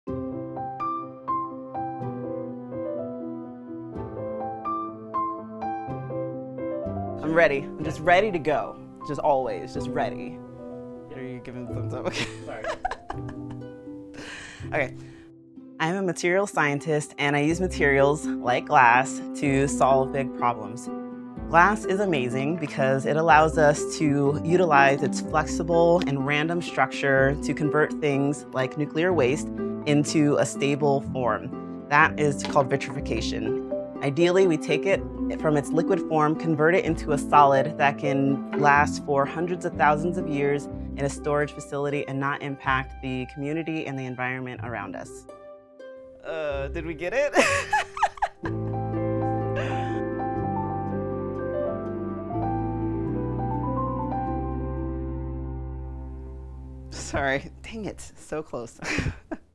Science Samples: Nuclear Waste to Glass
In Science Samples, researchers at Pacific Northwest National Laboratory describe a facet of their work—in 30 seconds! Materials scientist Charmayne Lonergan studies nuclear waste that has been turned into solid glass. The process, called vitrification, allows for safer storage.